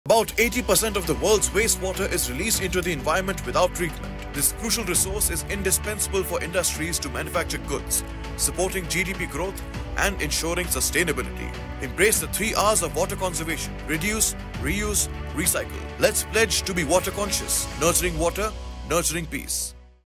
English Factoid 03 - World Water Day 2024
Join our CMD, Mr. Rajesh Sharma, for an insightful panel discussion on the importance of water conservation & management.

Watch "Nurturing Water, Nurturing Peace" live on Friday, 22 March 2024 at 6:30 pm on India Today TV